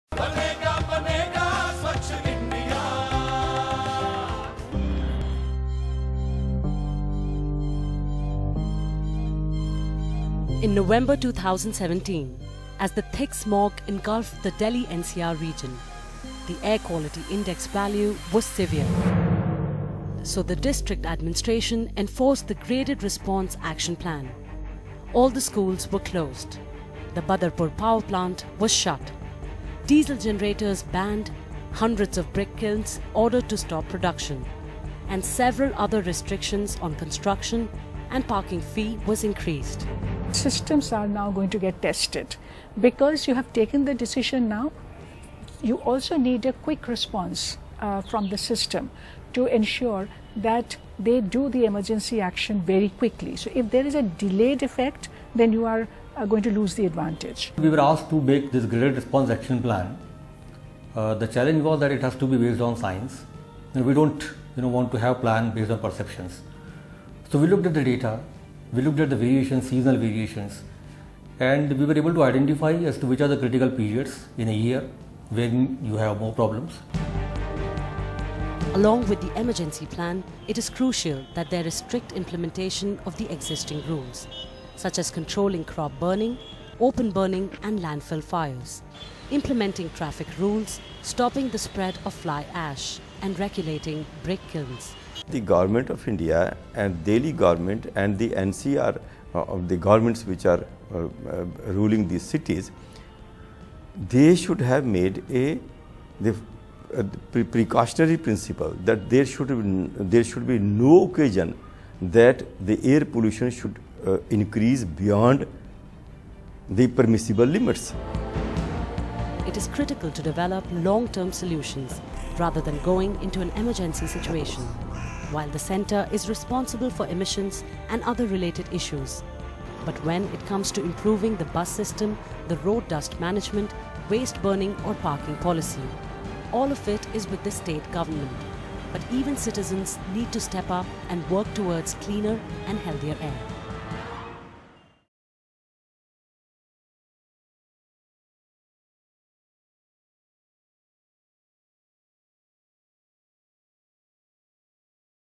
The Graded Response Action Plan, Delhi-NCR's Answer To Combat Air Pollution
While the Graded Response Action Plan notified by the Union Environment Ministry to combat air pollution was enforced in the national capital region, experts say it is crucial that there is strict implementation of existing rules for it to become effective.